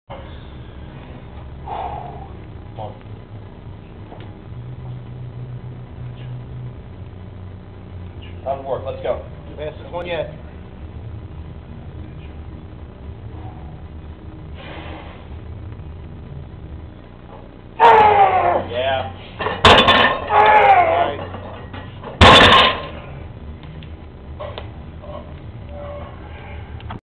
Niko Hulslander - Speed Pulls against Bands
455 pounds plus quaded minis. This is the last set of 8. Approx. 45 seconds rest. Getting ready for the USAPL PA State BP/DL on May 15th at Vision Fitness, Brogue, PA